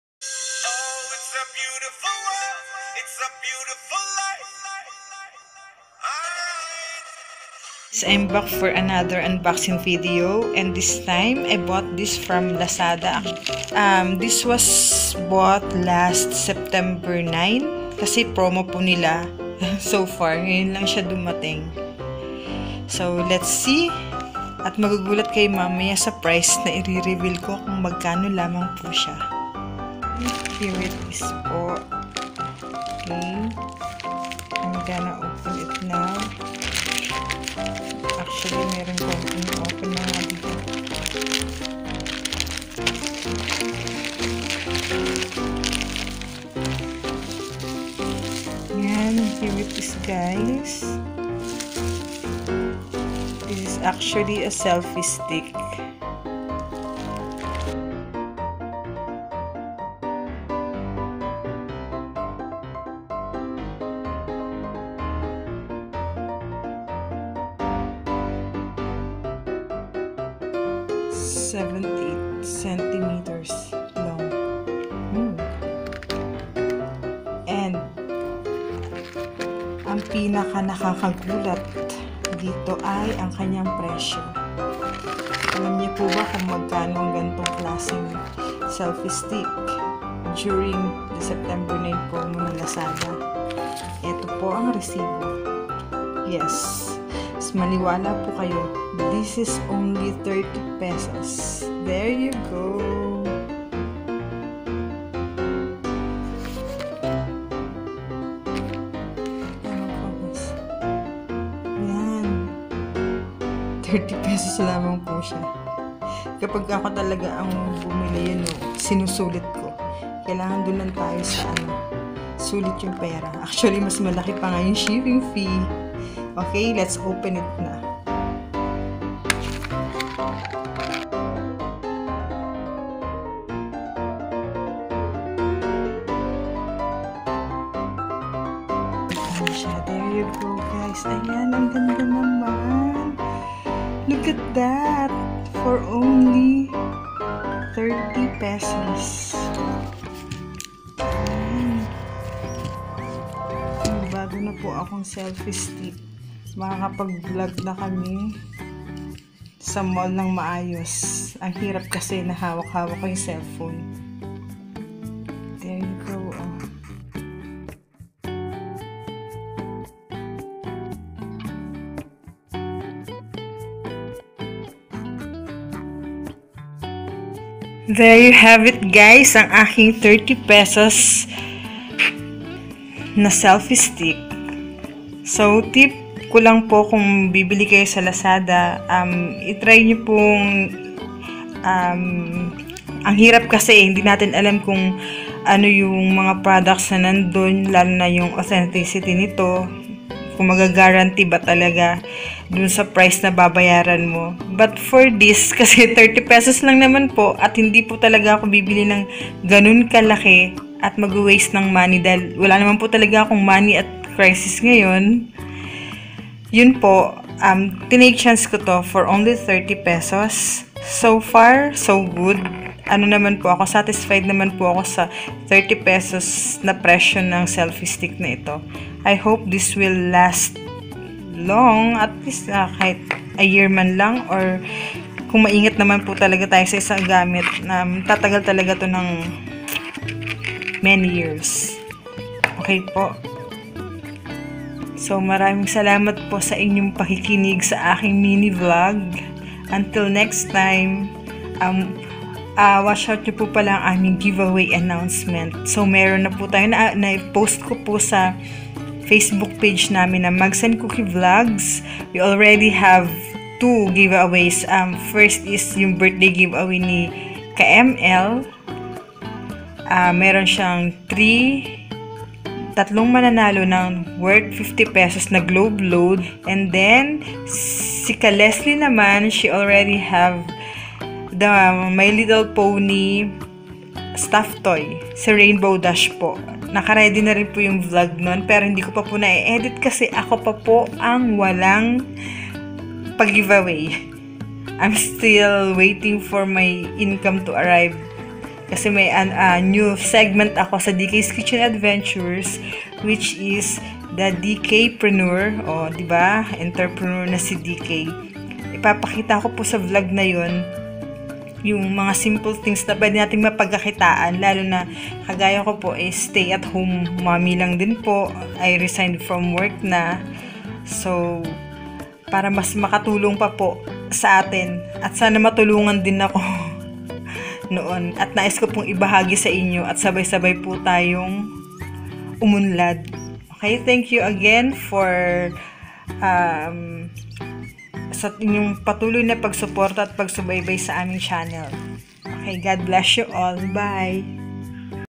9.9 Lazada Promo : Cheap Selfie Stick Unboxing
Hi guys!
We have here a new unboxing video, I get this item from Lazada during their 9.9 sale. Di ako gaanong mahilig sa online shopping, ika nga to each his own, but the price of this item caught my attention 😂